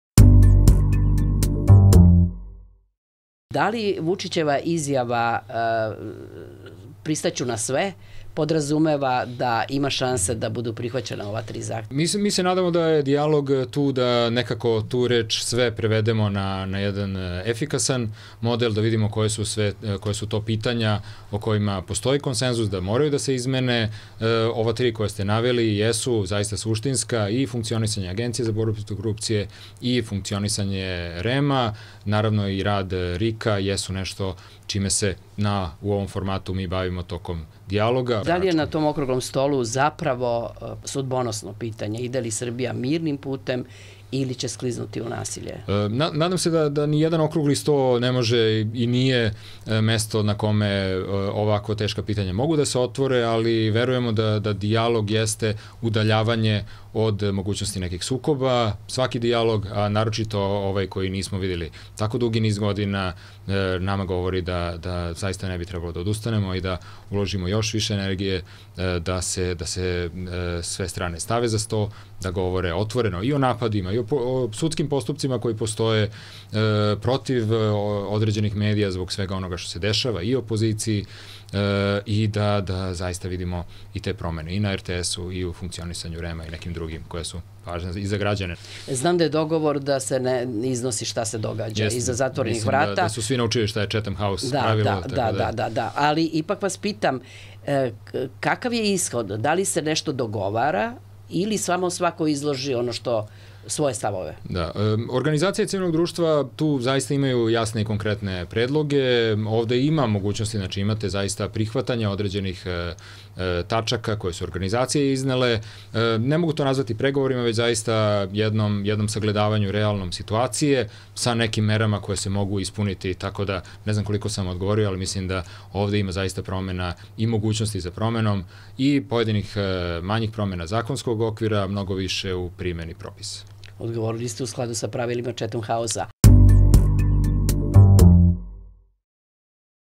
Antonijević: Tri zahteva Saveza za Srbiju o medijima su suštinska
U ponedeljak (19. avgust) na Fakultetu političkih nauka u Beogradu biće održan okrugli sto opozicije i vlasti o izbornim uslovima posvećen medijima.